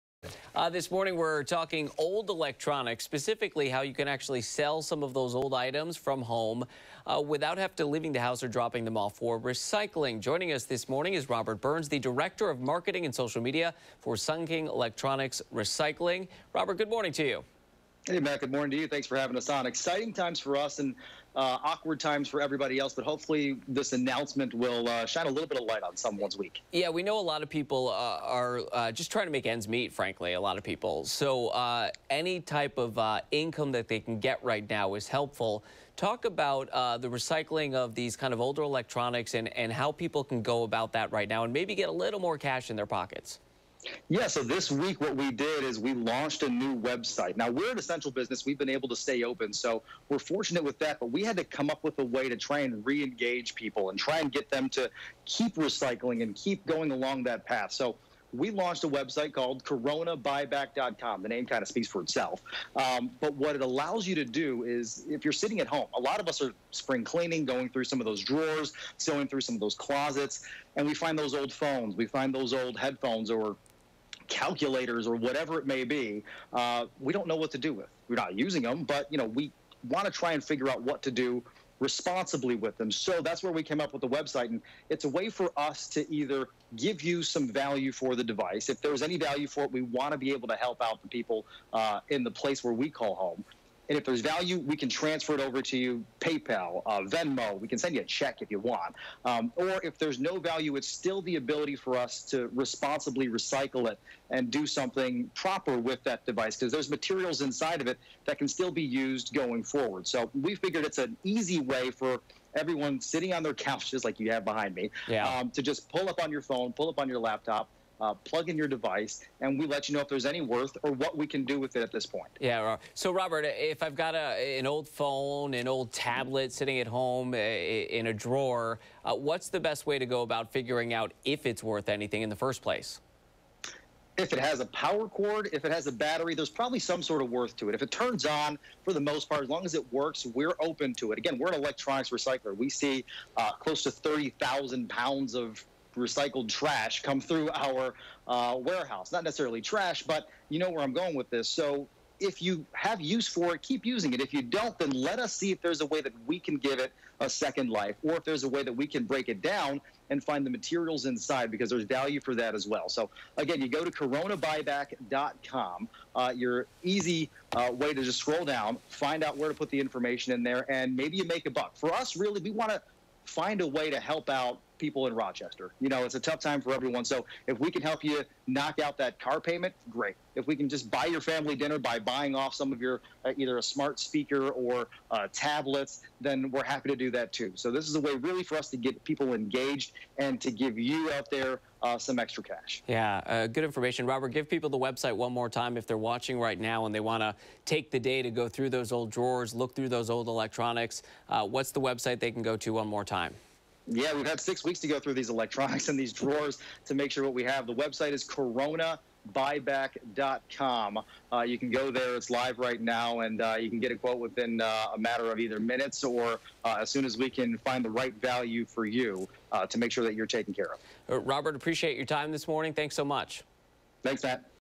(WUHF) Sunnking Electronics Recycling Launches Buyback Program
Sunnking Electronics Recycling launched CoronaBuyBack.com in hopes its new recycling initiative helps people amid the coronavirus pandemic.

The organization is allowing people to electronically submit old tech they’d like to have recycled. Sunnking will help coordinate an appraisal, and users could receive payment via check or online payment service.

Director of Marketing and Social Media, Robert Burns, joins us with more details.